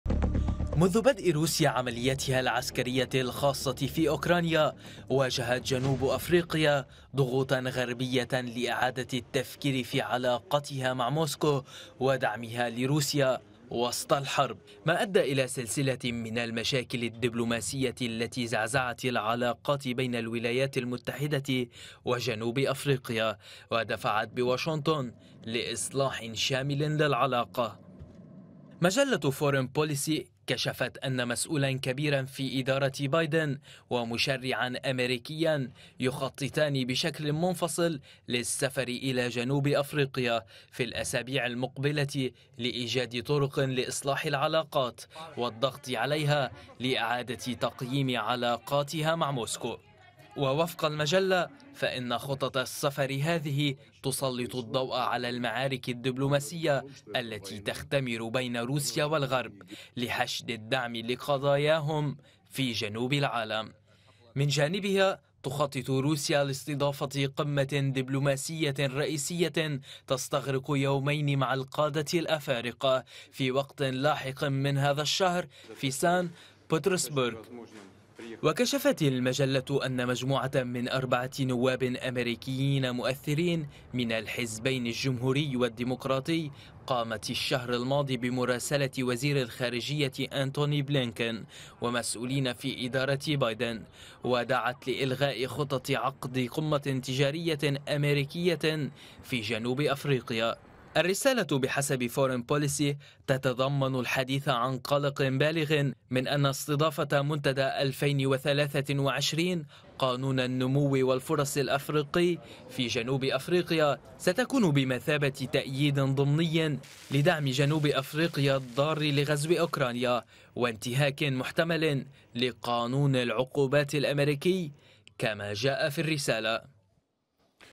جهود اميركية لإصلاح العلاقة مع جنوب أفريقيا
كشفت مجلة فورين بوليسي أن مسؤولاً كبيراً في إدارة بايدن ومشرعاً أميركياً يخططان للسفر إلى جنوب أفريقيا في الأسابيع المقبلة لإيجاد طرق لإصلاح العلاقات بين البلدين والضغط على كيب تاون لإعادة تقييم علاقتها مع موسكو.

إعداد: محمد بزي